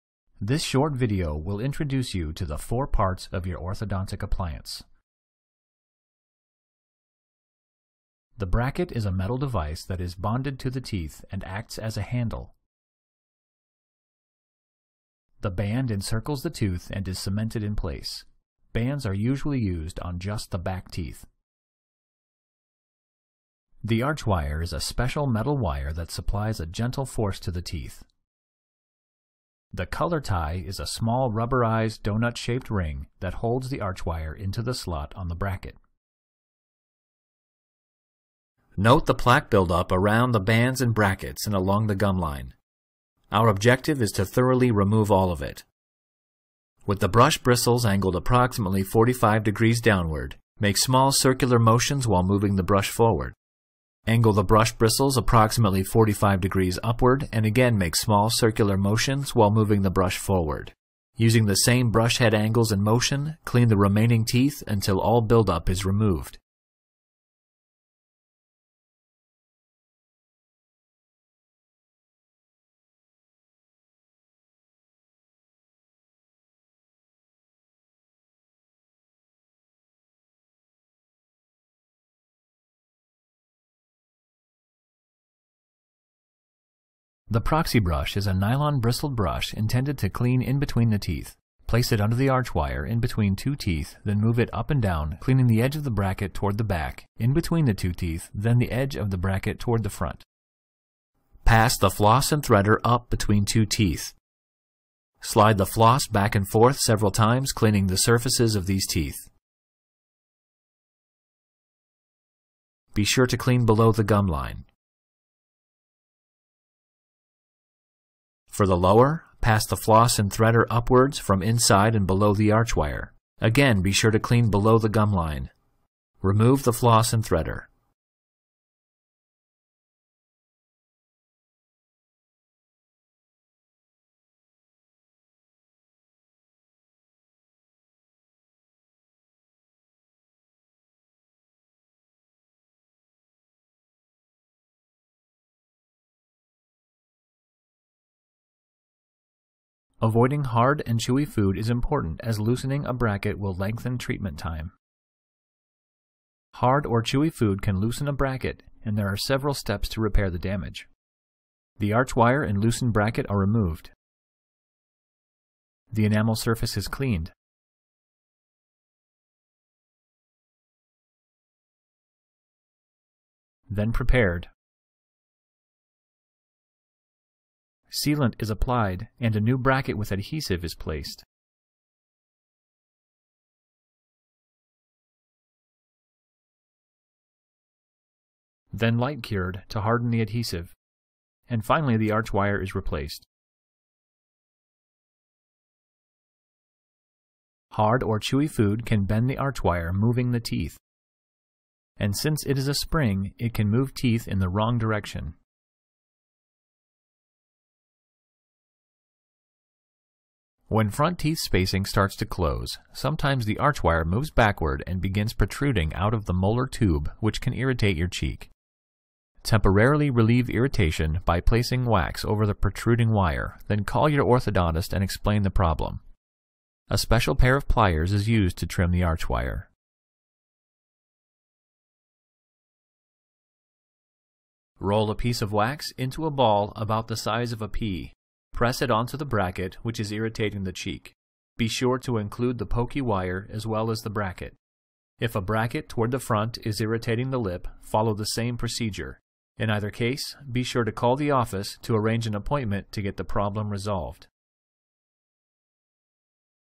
Braces Care and Information
This video will aid you in how to care for your braces.